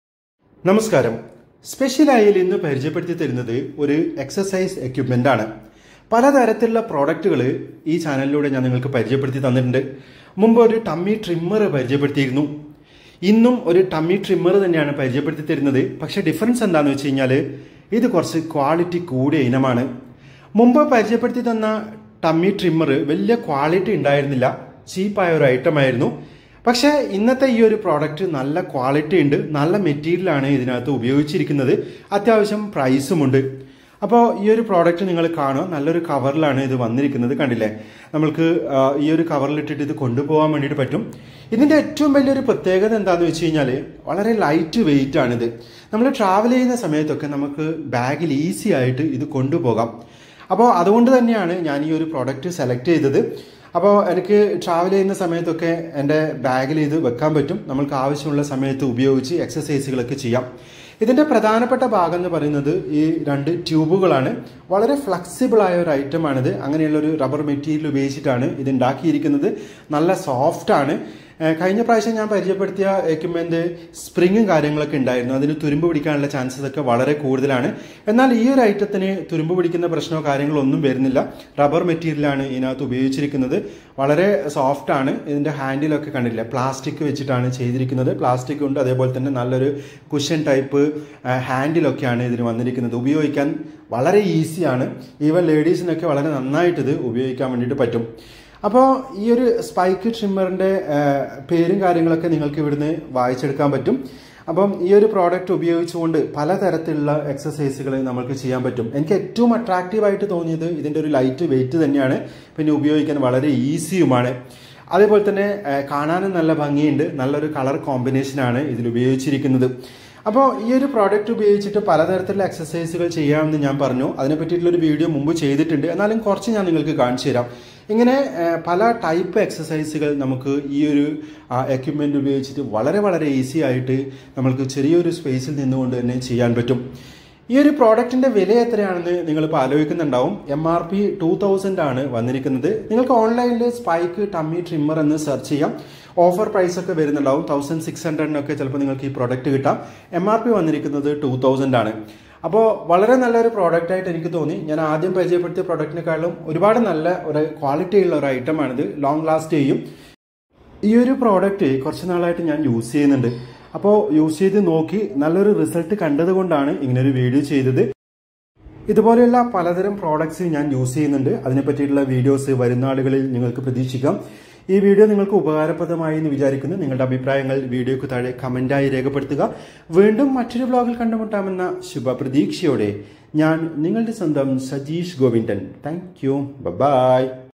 ഇത് വാങ്ങിയാൽ കാശ് നഷ്ടമാകില്ല /Spike Tummy Trimmer
Spike tummy Trimmer exercise equipment review by Sajeesh Govindan